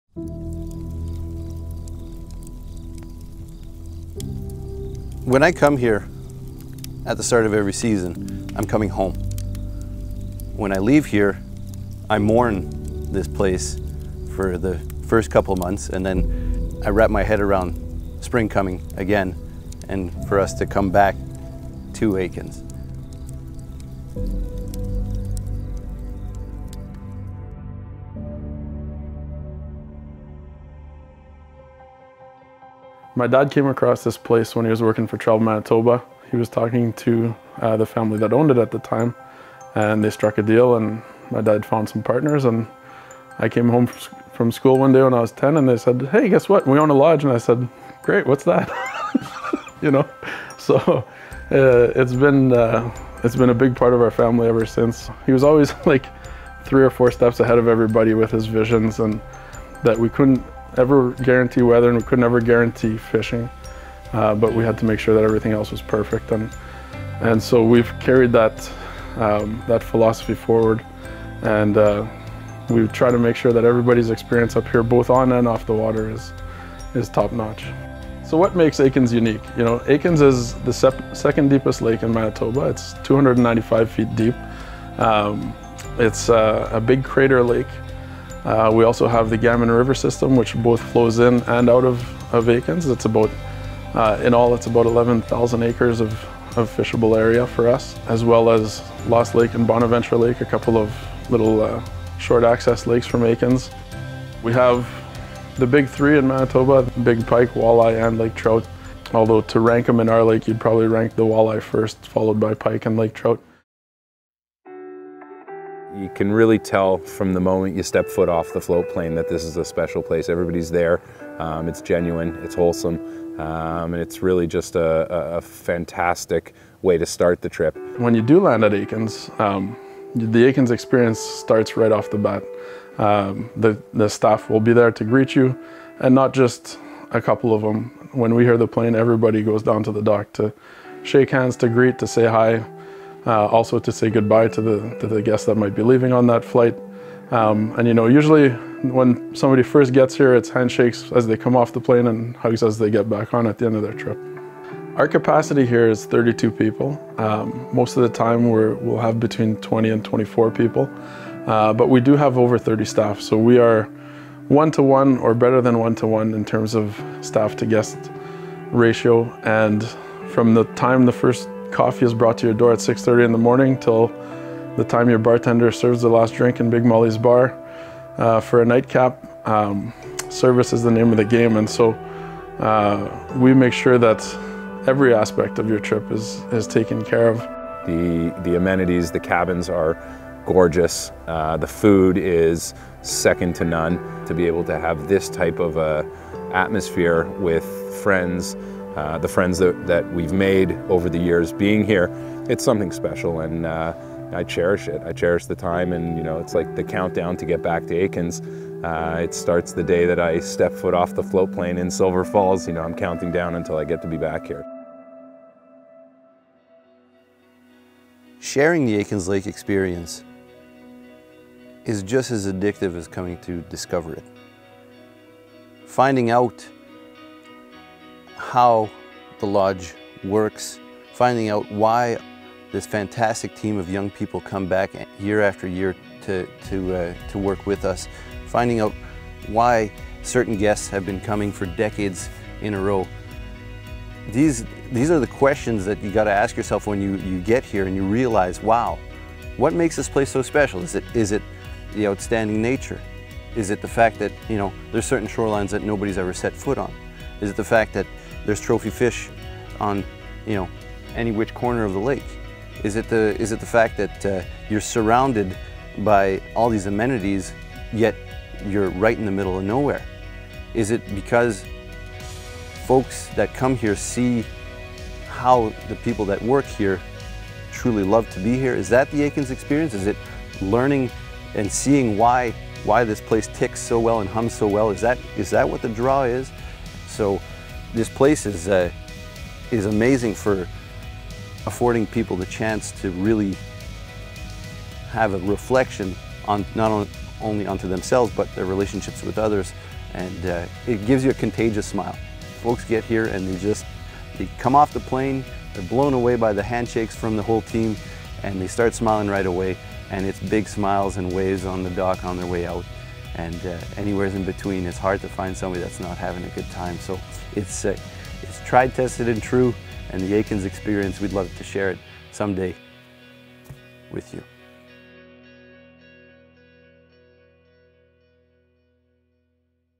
'The Aikens Experience'- Manitoba Fly-In Fishing at Aikens Lake Wilderness Lodge
Manitoba Destination Spotlight: Manitoba Fly-In Fishing at Aikens Lake Wilderness Lodge.

Established as one of the top fly-in fishing lodges in Manitoba, Aikens Lake Wilderness Lodge has a rich and deeply rooted history. Developed in 1949, Aikens sits within the boundaries of Atikaki Provincial Park and amongst the wild expanses of Manitoba’s Eastern Region. Their long-standing dedication to providing a unique and luxurious angling experience set in a pristine and breathtaking location has earned them recognition as being A World’s Foremost Destination.

For more information on fishing at Aikens Lake Wilderness Lodge, visit their